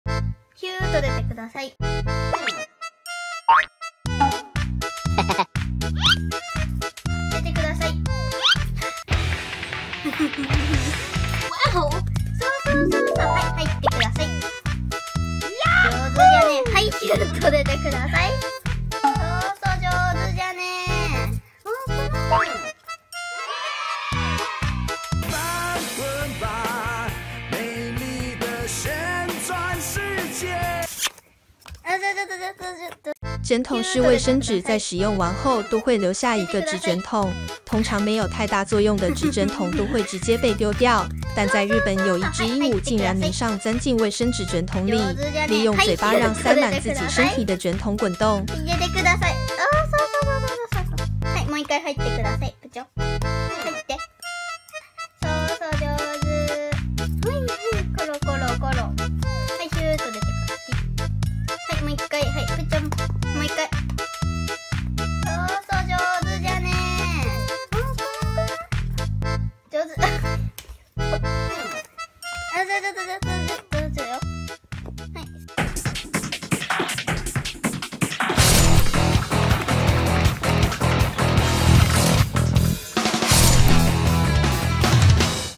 寵物鸚鵡鑽捲筒竟變代步工具 嘴巴靈活控速快速翻滾｜三立娛樂星聞
AP／Jukin Media 授權